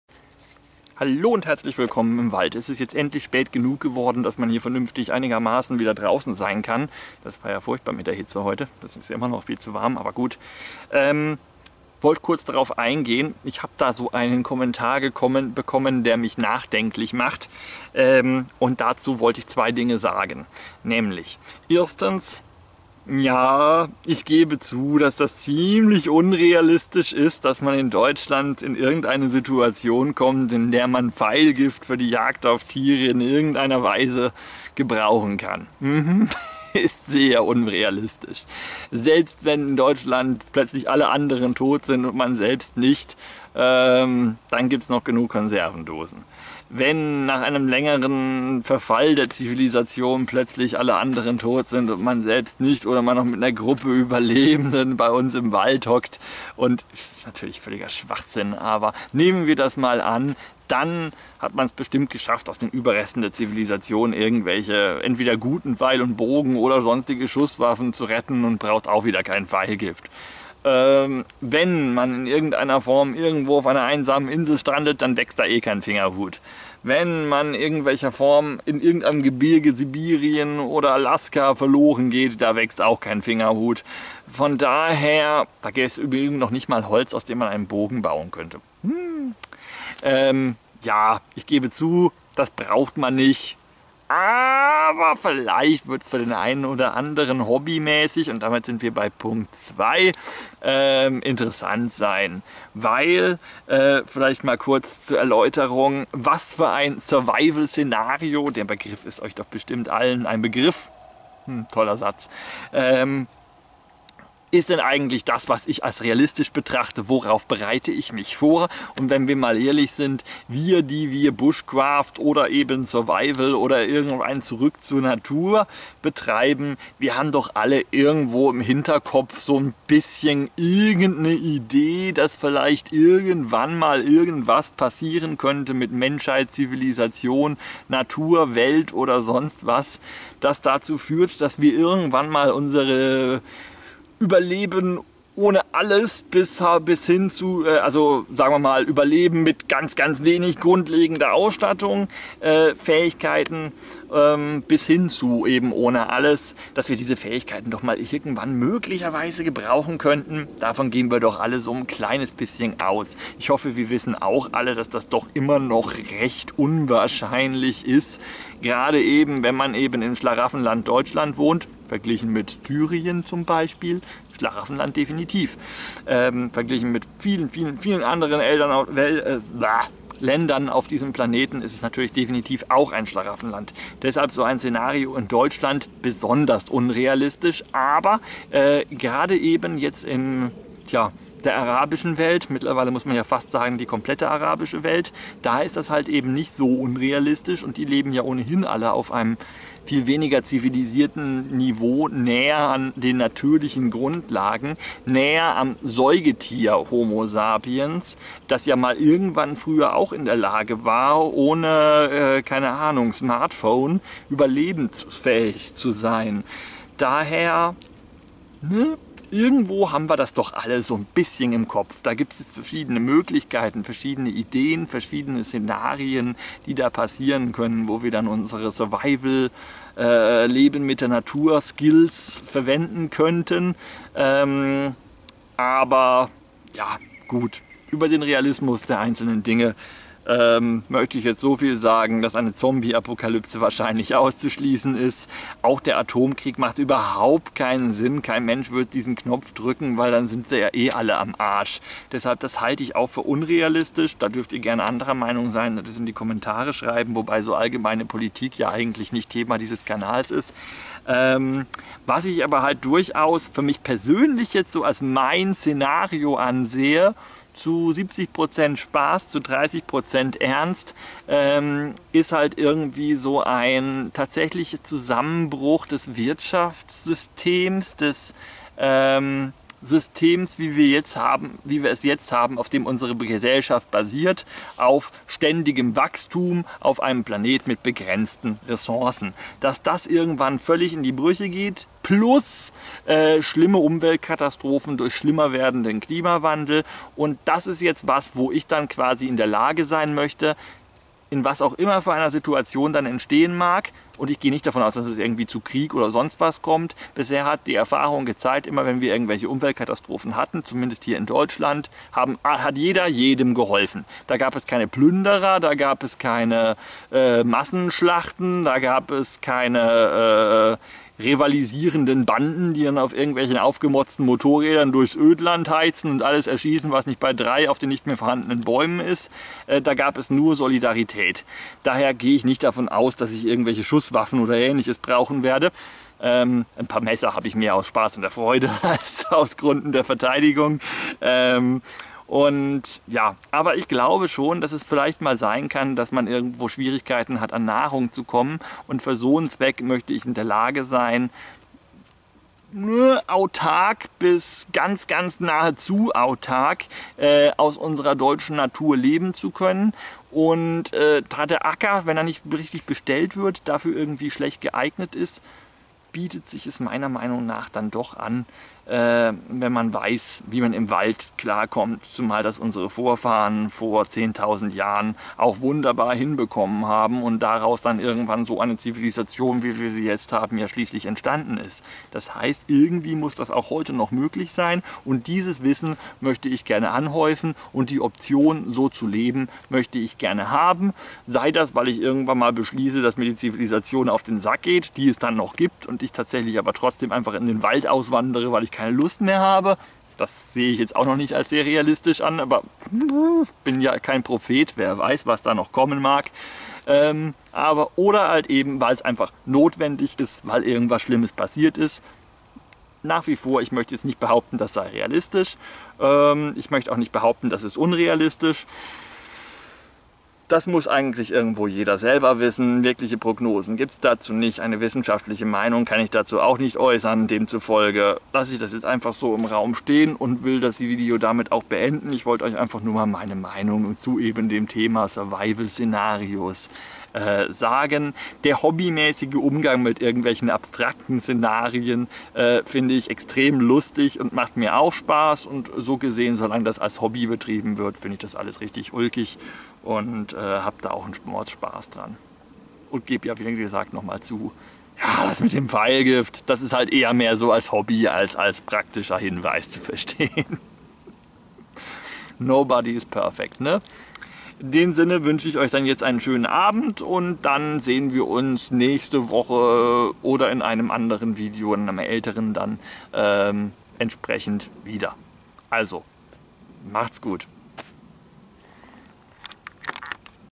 Wald-Fragestunde #11 - Wozu soll Pfeilgift gut sein? Survival-Szenarien
Kaum erzählt man mal ein bisschen Mist, kommt auch schon ne Frage dazu! :-D

Vielleicht habt ihr spaß an meiner extra kurz gehaltenen, persönlichen Stellungnahme zu Sinn und Unsinn unser aller Wald-Hobbys! :-)